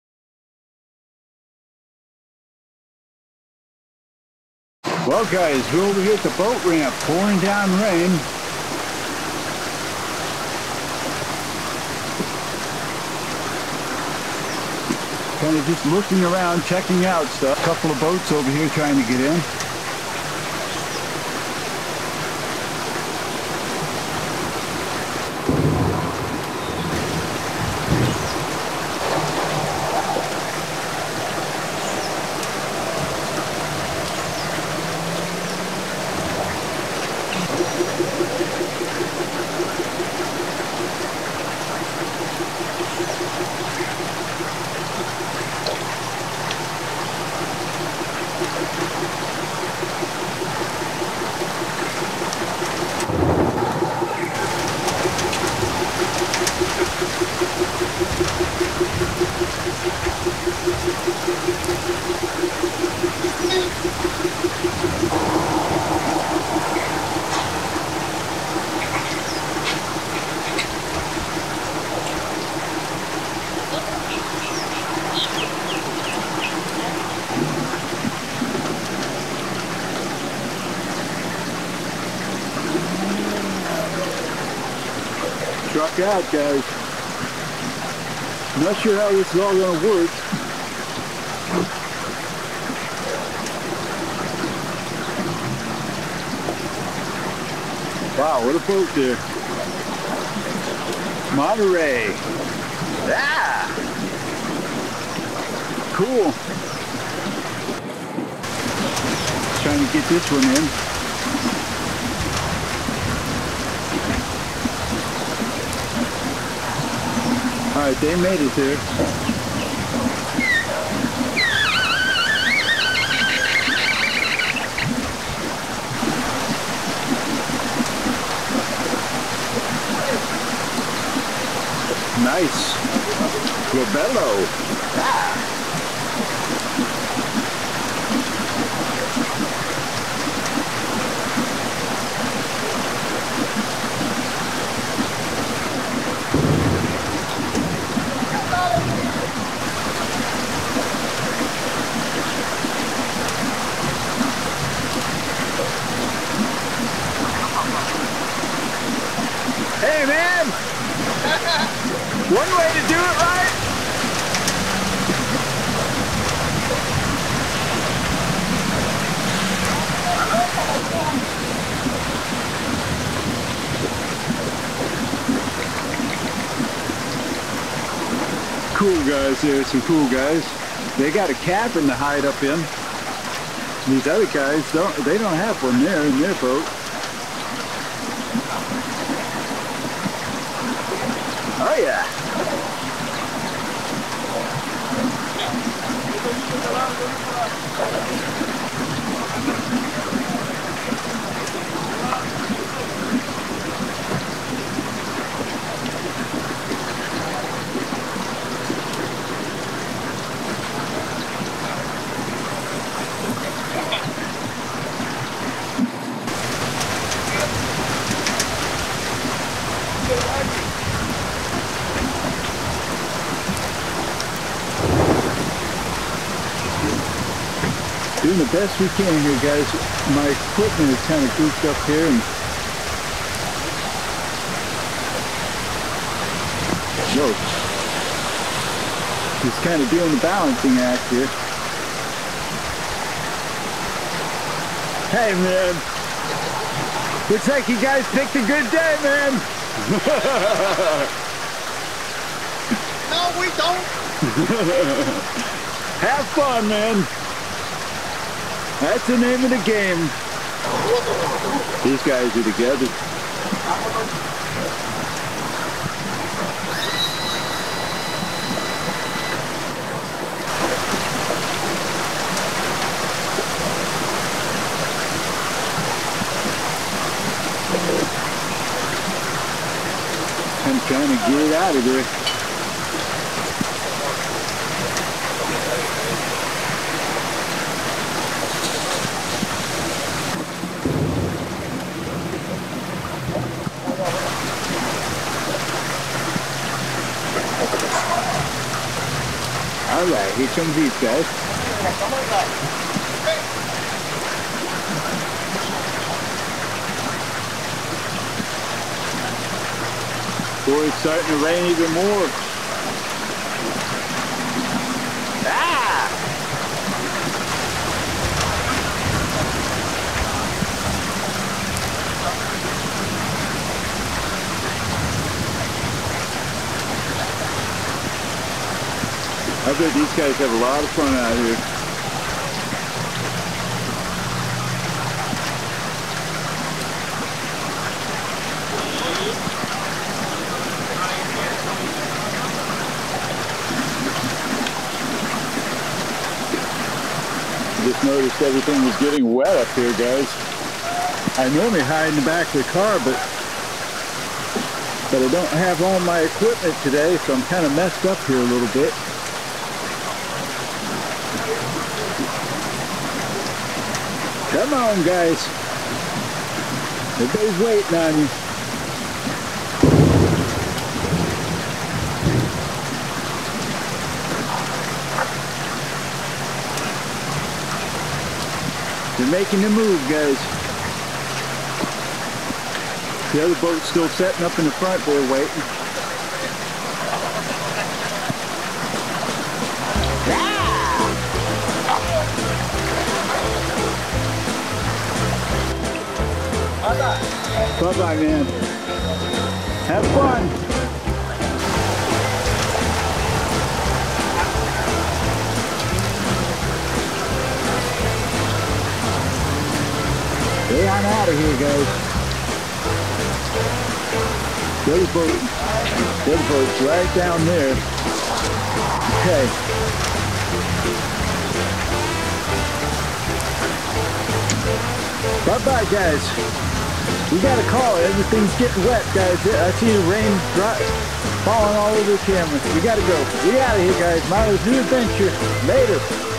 Storming At The Boat Ramp Two Big Boats Trying To Get In Crazy
Early Moring at the Boat Ramp bad a bad Thunderstorm. Two big boats trying to get
into the water raining real bad hang on Grab a snack and Enjoy.